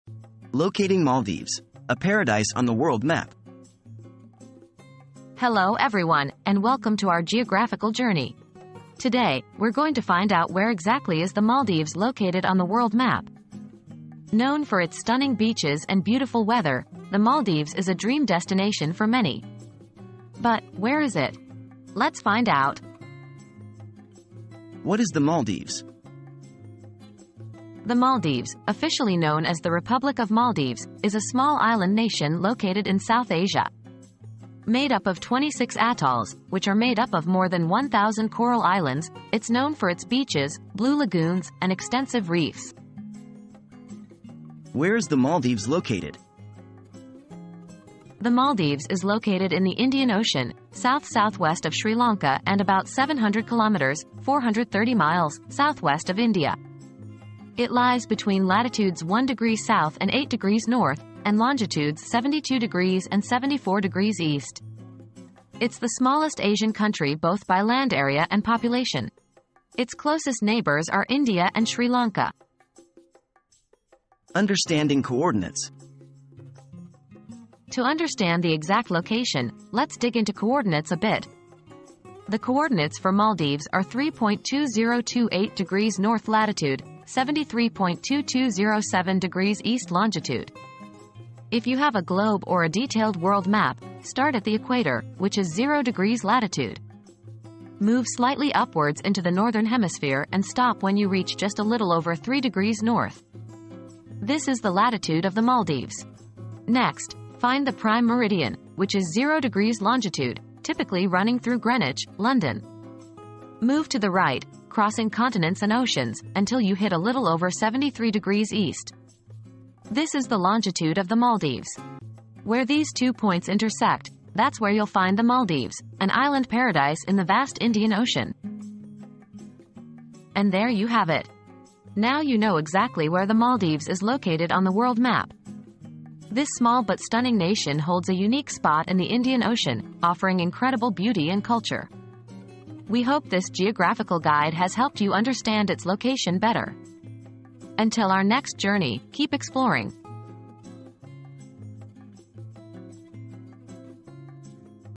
Locating Maldives: A Paradise on the World Map
What is the exact location of Maldives on the world map? • Discover the exact location of the Maldives, a paradise on the world map! Join us on a geographical journey as we explore the coordinates, latitude, and longitude of this stunning island nation. From its 26 atolls to its blue lagoons and extensive reefs, the Maldives is a dream destination for many. Let's find out where it is located and uncover the beauty of this unique spot in the Indian Ocean!

00:00 • Locating Maldives: A Paradise on the World Map
00:24 • What is the Maldives?
00:44 • Where is the Maldives Located?
01:14 • Understanding Coordinates